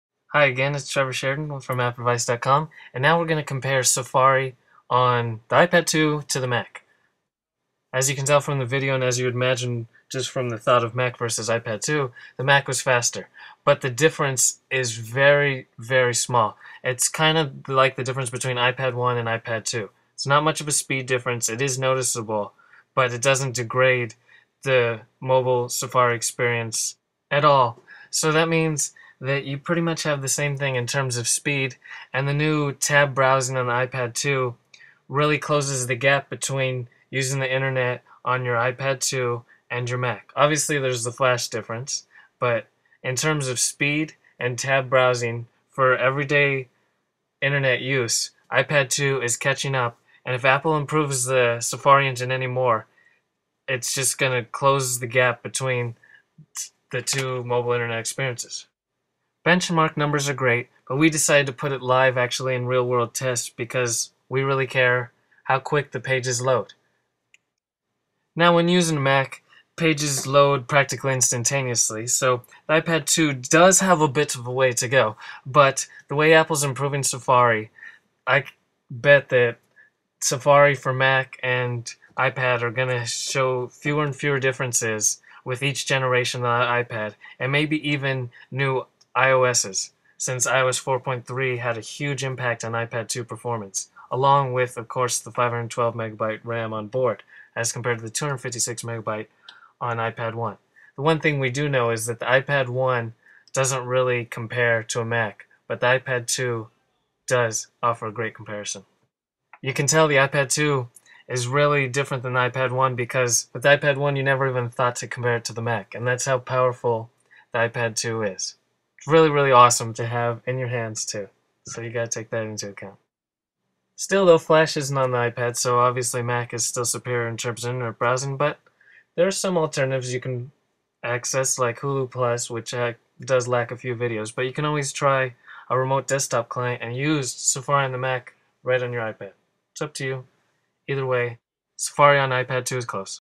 iPad 2 vs. Mac Safari Perfrormance
iPad 2 Safari goes head to head with a Mac running Safari.